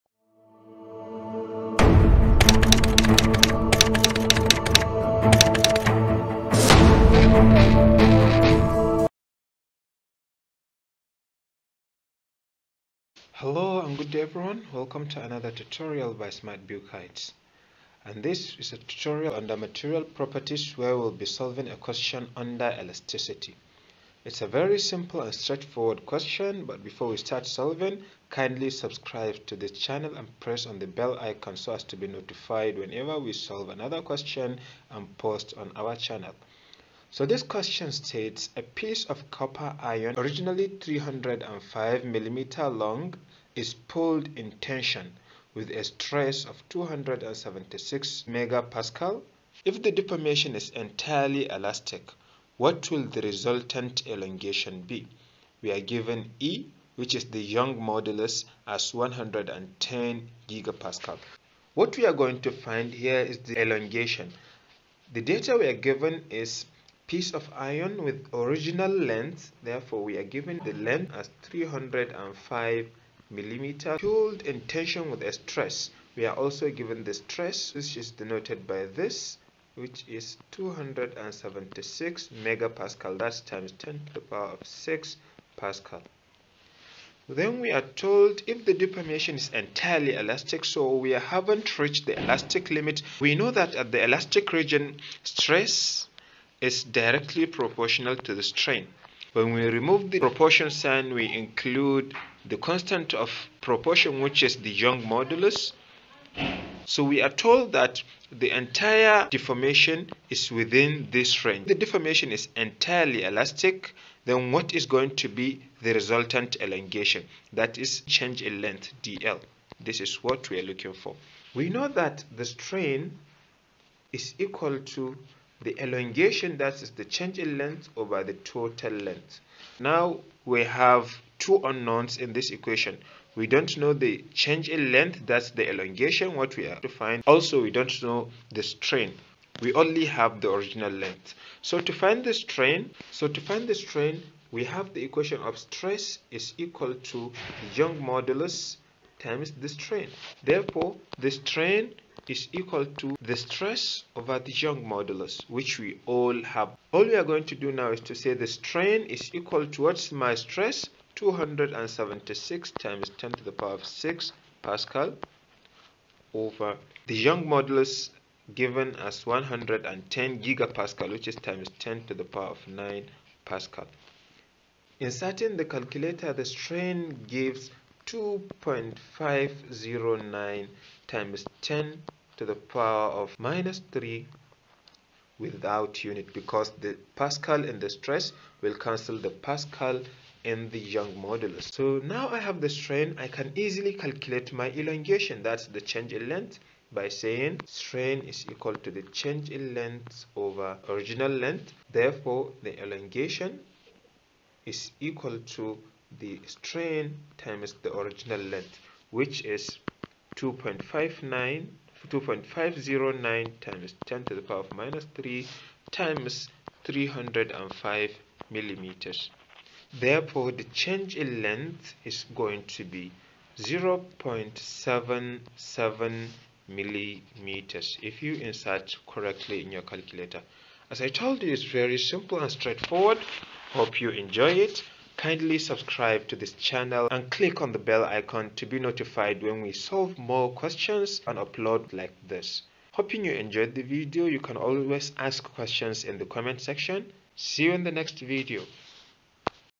What will the resultant elongation be? if E=110 GPa, Stress is 276 Mpa and wire length is 305mm.
This is a solution to a question under Modulus of Elasticity, The question is
: A piece of copper originally 305mm long is pulled in tension with a stress of 276 MPa. If the deformation is entirely elastic, what will the resultant elongation be? E=110 GPa.